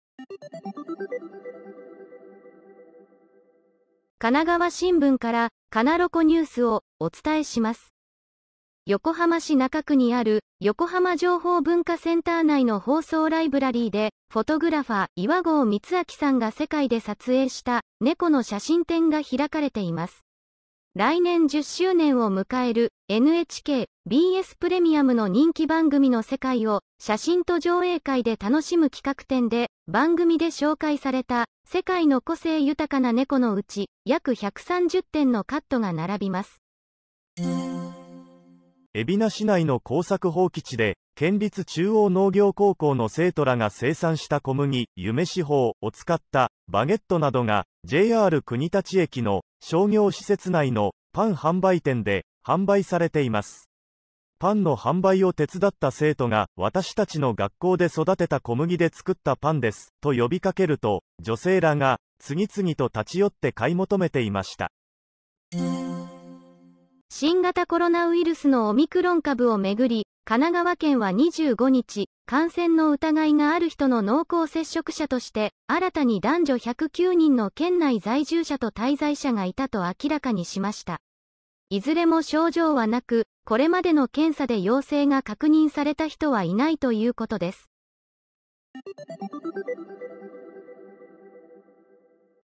横浜で「世界ネコ歩き」写真展 AIアナ・１２月２６日／神奈川新聞（カナロコ）
神奈川新聞から「カナロコニュース」をお伝えします。
・横浜で「世界ネコ歩き」写真展（横浜市）
・「中央農業高産小麦」のパン好評（海老名市）
・オミクロン濃厚接触者１０９人（神奈川県）